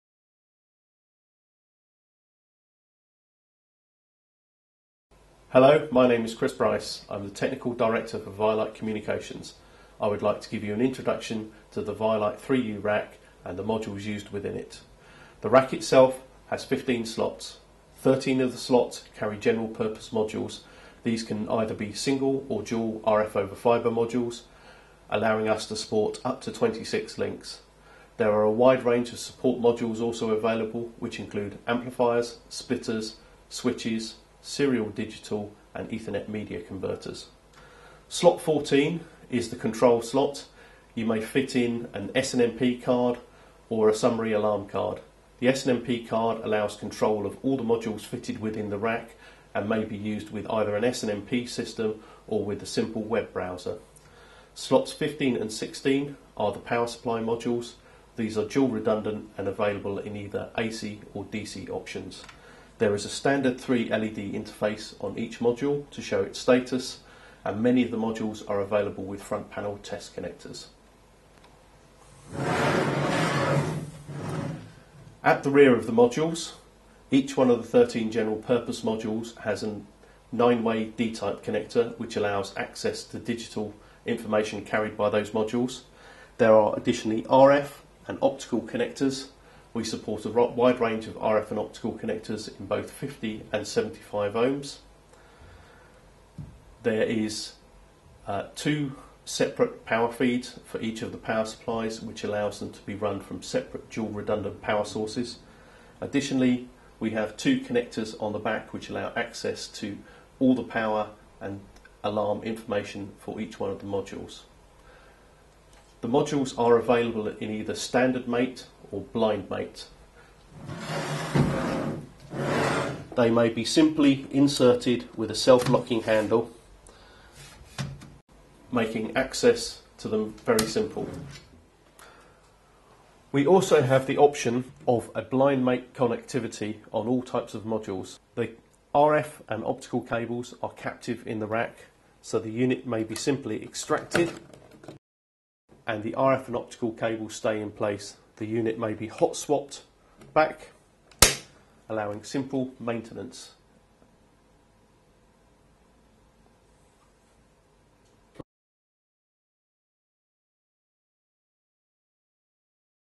ViaLite HD 3U rack demo
Demonstration of ViaLiteHD 3U rack by Chris Brice, technical director of ViaLite Communications.